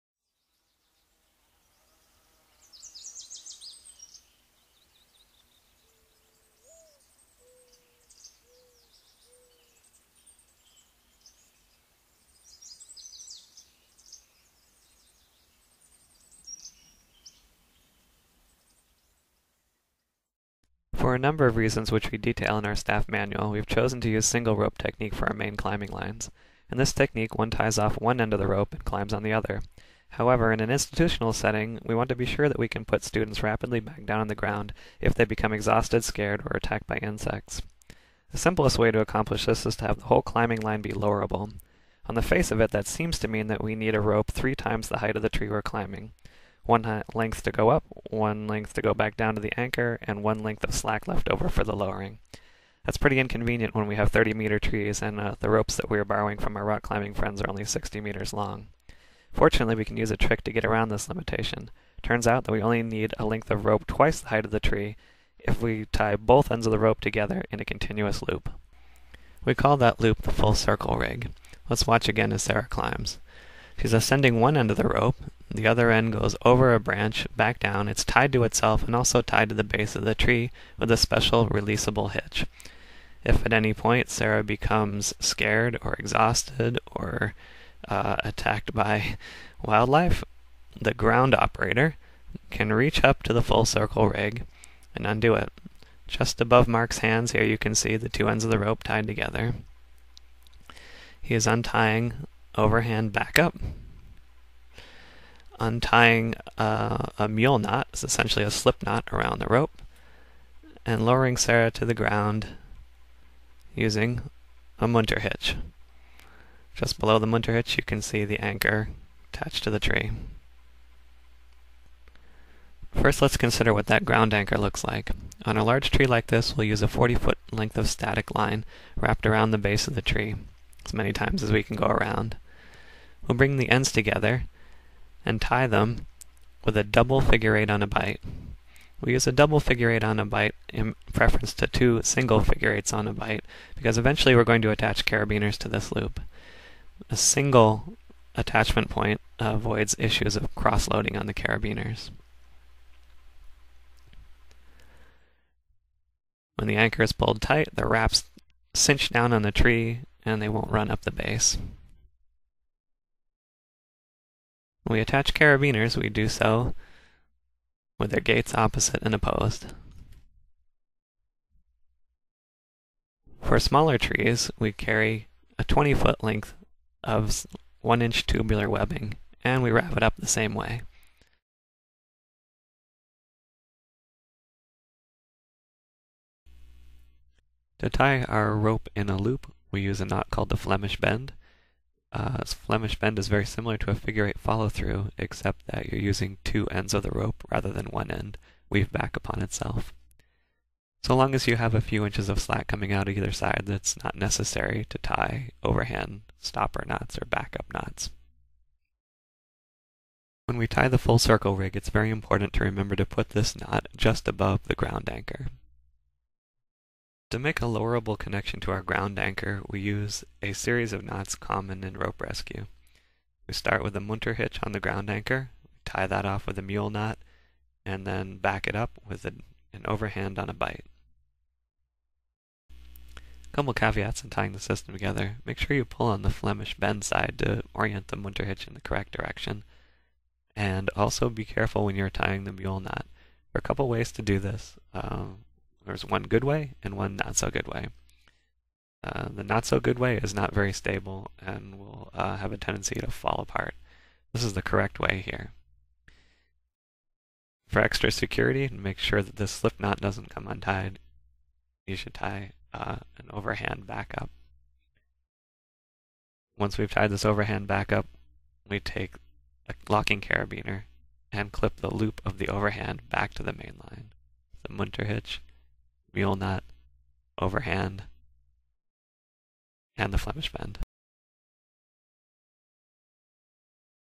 Tree Climbing: Ascending with the Full Circle Rig
This videos illustrates one of the ascending techniques used in the Cornell Tree Climbing Institute's instructional classes.  This information is not intended to be an online tutorial for beginners.  Our intent is to share these general procedures with other qualified professionals who are interested in developing a college-based recreational tree climbing program.  Tree climbing can be hazardous!  If you are new to tree climbing,  seek qualified instruction before attempting these techniques.